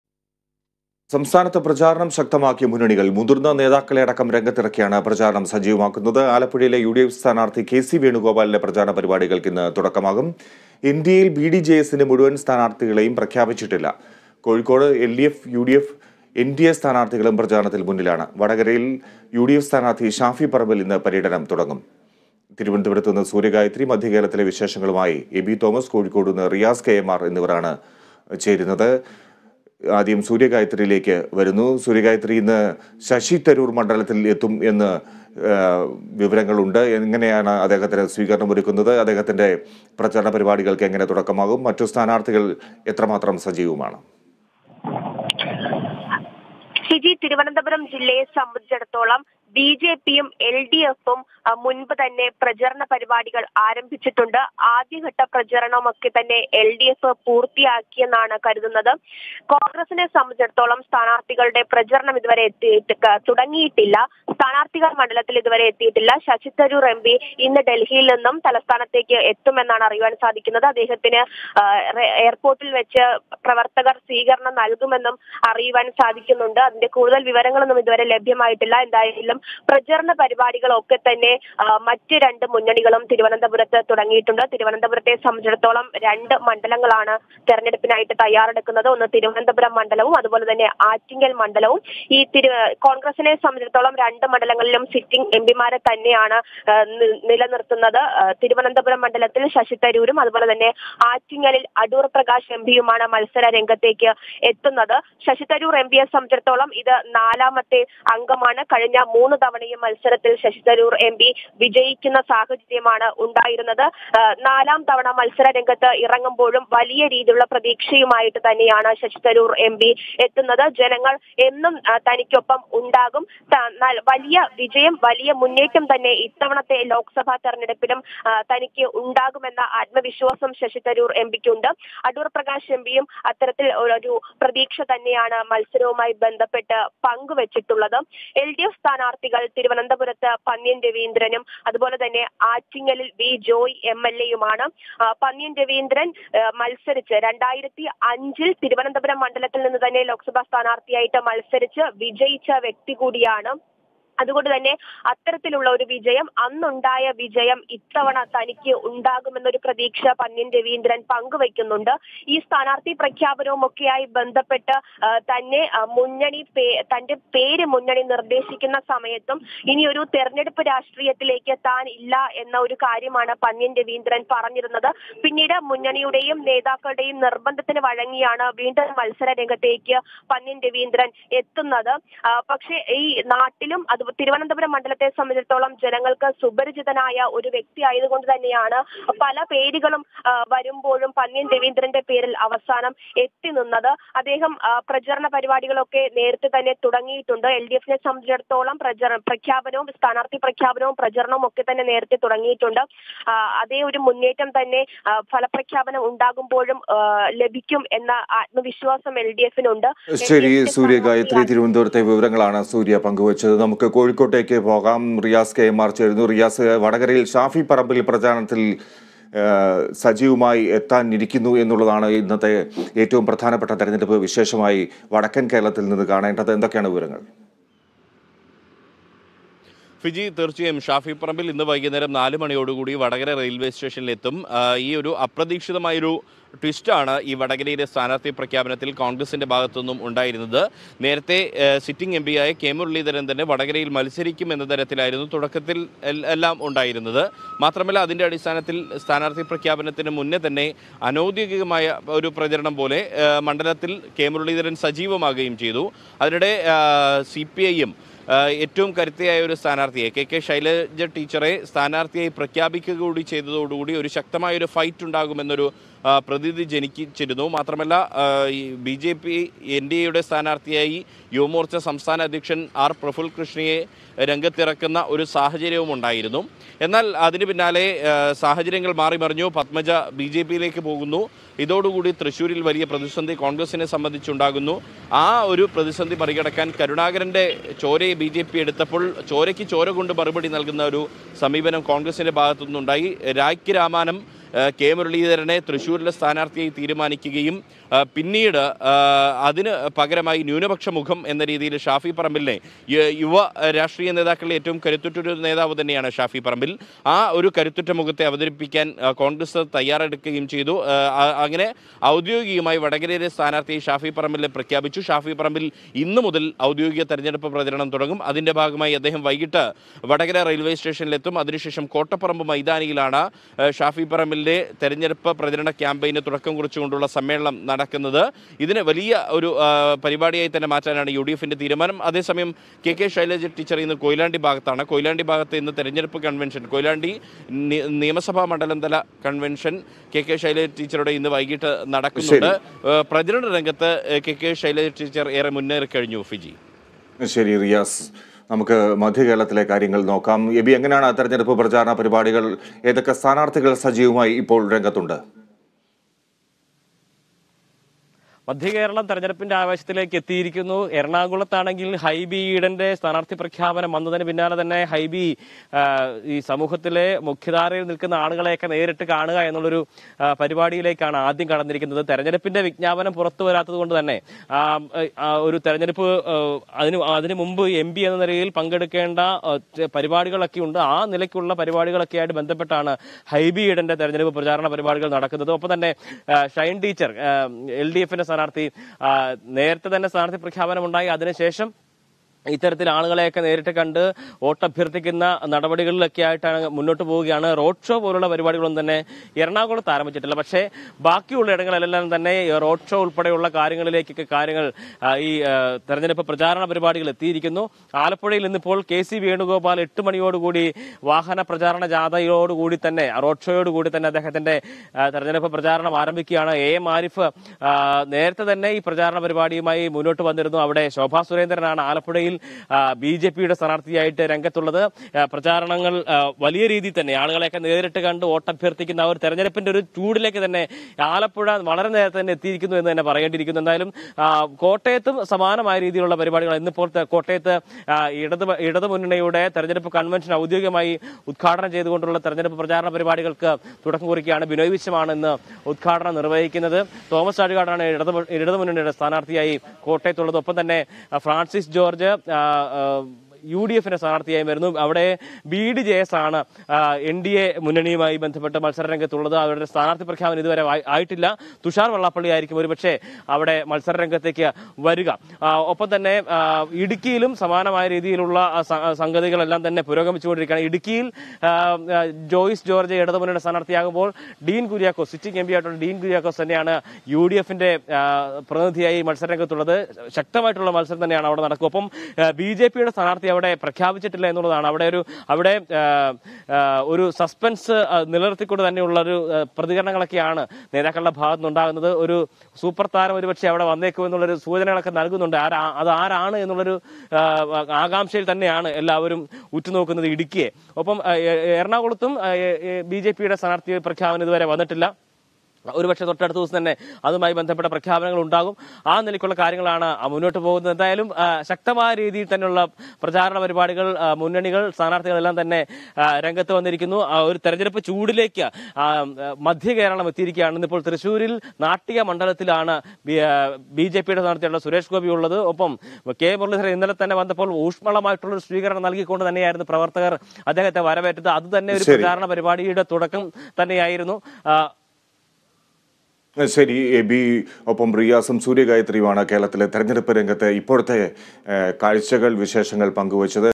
സംസ്ഥാനത്ത് പ്രചാരണം ശക്തമാക്കി മുന്നണികള്‍;മുതിര്‍ന്ന നേതാക്കളടക്കം പ്രചരണ രംഗത്ത് സജീവം
സംസ്ഥാനത്ത് പ്രചാരണം ശക്തമാക്കി മുന്നണികള്‍. മുതിര്‍ന്ന നേതാക്കളെയടക്കം രംഗത്തിറക്കിയാണ് പ്രചരണം സജീവമാക്കുന്നത്. ആലപ്പുഴയിലെ യുഡിഎഫ് സ്ഥാനാര്‍ഥി കെ.സി വേണുഗോപാലിന്റെ പ്രചരണ പരിപാടികള്‍ക്ക് ഇന്ന് തുടക്കമാകുകയാണ്. എന്‍ഡിഎയില്‍ ബിഡിജെഎസിന്റെ മുഴുവന്‍ സ്ഥാനാര്‍ഥികളെയും പ്രഖ്യാപിച്ചിട്ടില്ല.

About COA